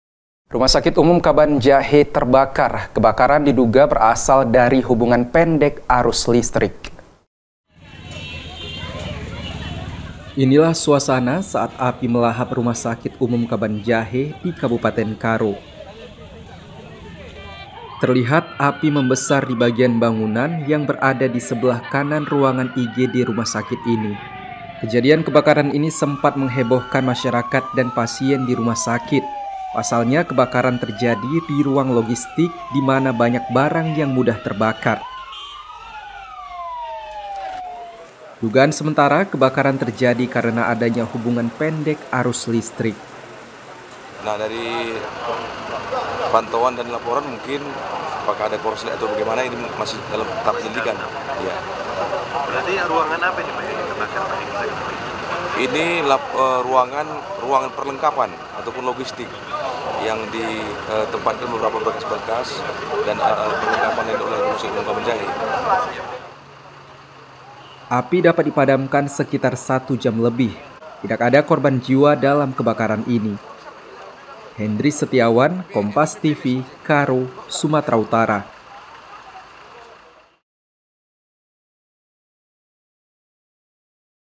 Rumah Sakit Umum Daerah Kabanjahe Terbakar
KARO, KOMPAS.TV - RSUD Kabanjahe terbakar pada Senin (28/11).

Terlihat api membesar di bagian bangunan yang berada di sebelah kanan ruangan IGD.

Kejadian kebakaran ini sempat menghebohkan masyarakat dan pasien di rumah sakit.

Dugaan sementara, kebakaran terjadi karena adanya hubungan pendek arus listrik. (*)